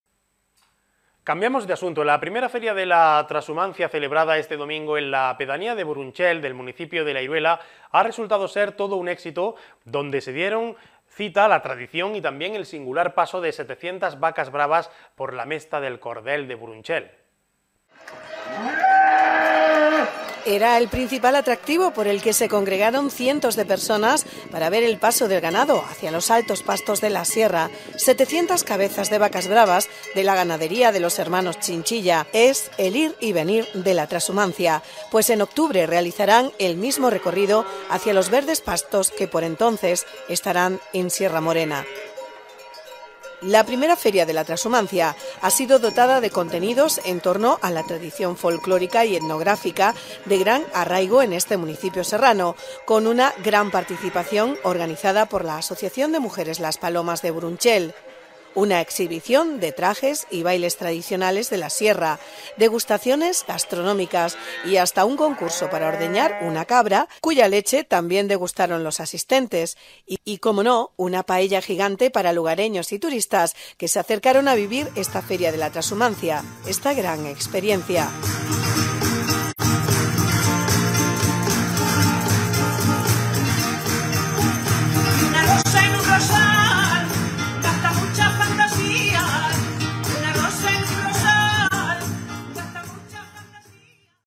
La Iruela - I Feria de la Trashumancia (5-06-23)
La primera feria de la trashumancia celebrada este domingo en la pedanía de Burunchel del municipio de La Iruela ha resultado ser todo un éxito, donde se dieron cita folclore y tradición y el espectacular paso de 700 vacas bravas por la mesta del cordel de Burunchel.